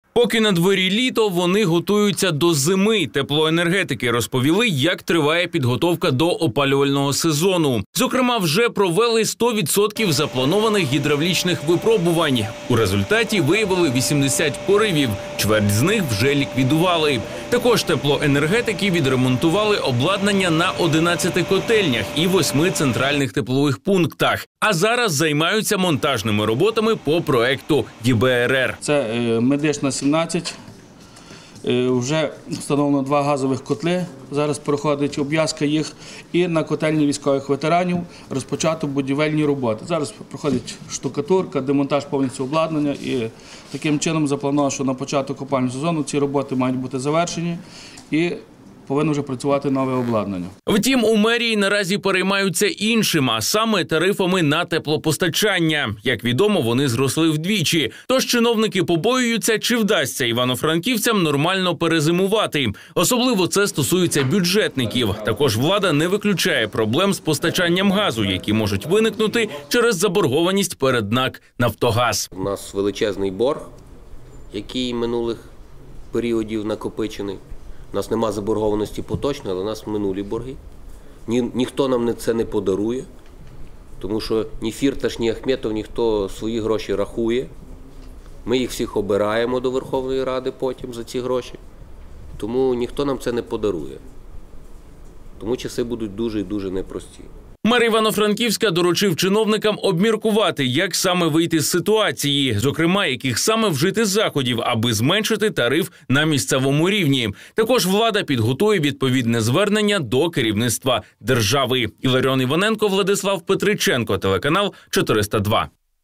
Франківські тепловики вже перевірили мережі і ремонтують опалення на котельнях
Поки на дворі літо — вони готуються до зими. Теплоенергетики розповіли, як триває підготовка до опалювального сезону.